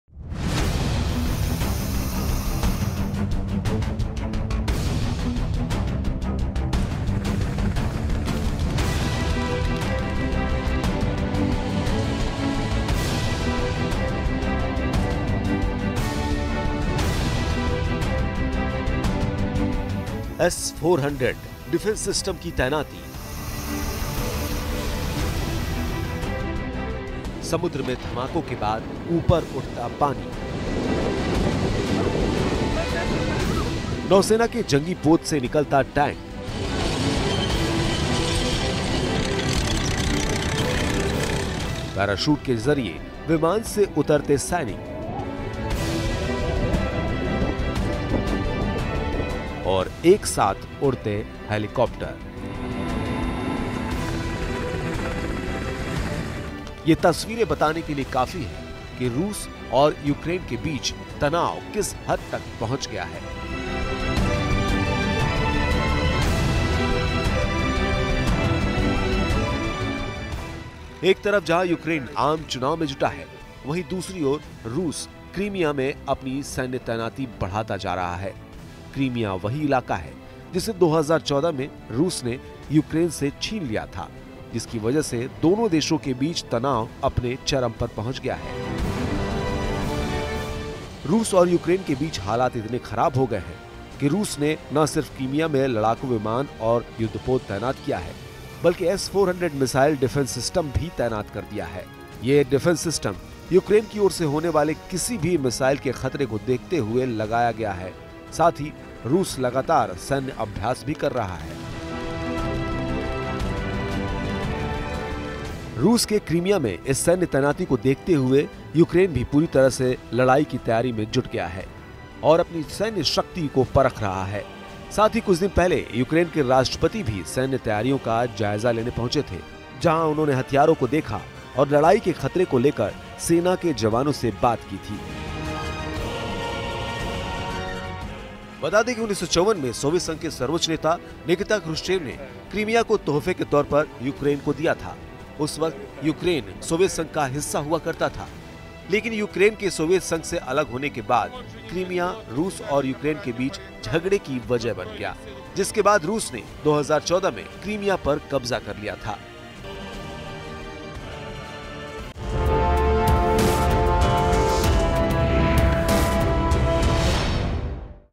रूस ने यूक्रेन बॉर्डर पर भेजे S-400 हथियार अमेरिका परेशान ! | Duniya Tak
रूस ने यूक्रेन बॉर्डर पर भेजे S-400 हथियार अमेरिका परेशान !.. देखते रहें दुनिया तक..

Russia sent S-400 on Ukrainian border.. US worried about this russian step.. Keep watching Duniya Tak..